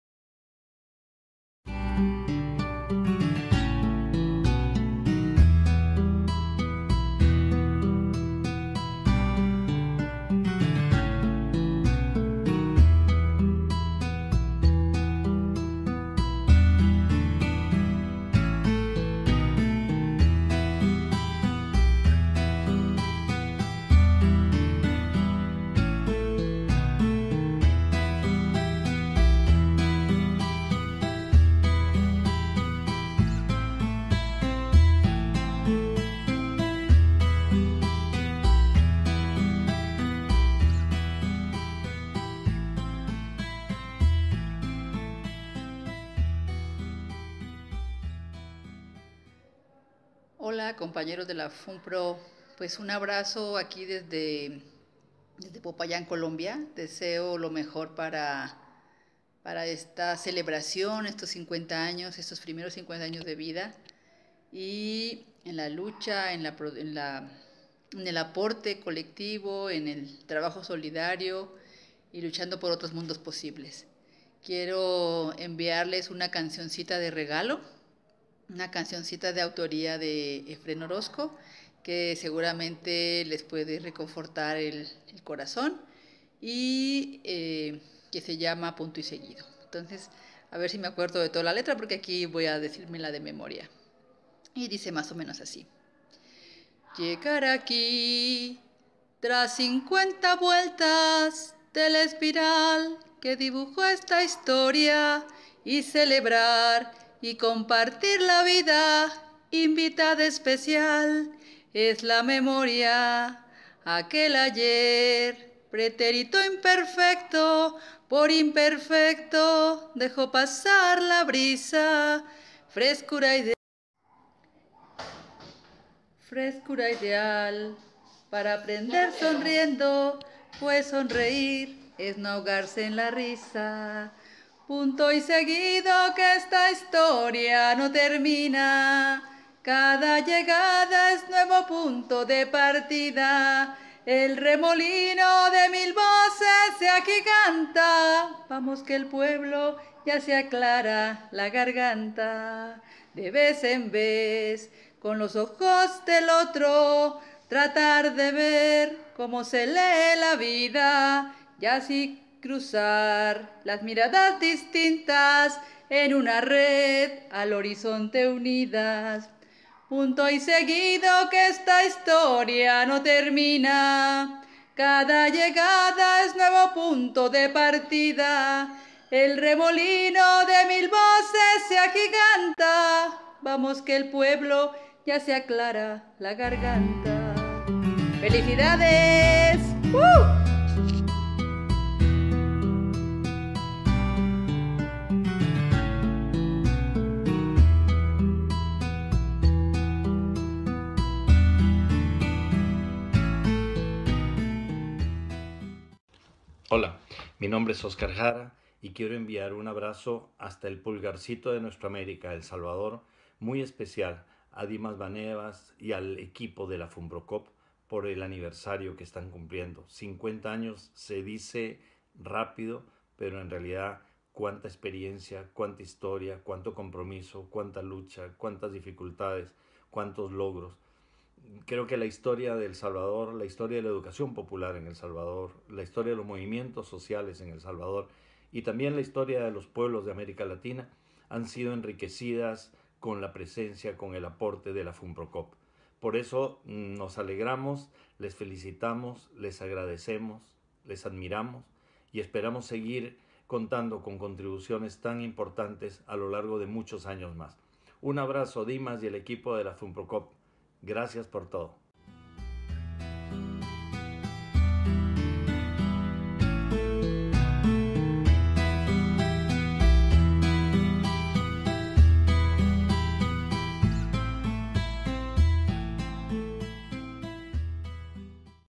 SALUDOS A FUNPROCOOP POR SUS 50 AÑOS
Miembros de los centros de la Red Alforja envían sus saludos a la Fundación Promotora de Cooperativas (Funprocoop) con motivo de celebrar sus 50 años.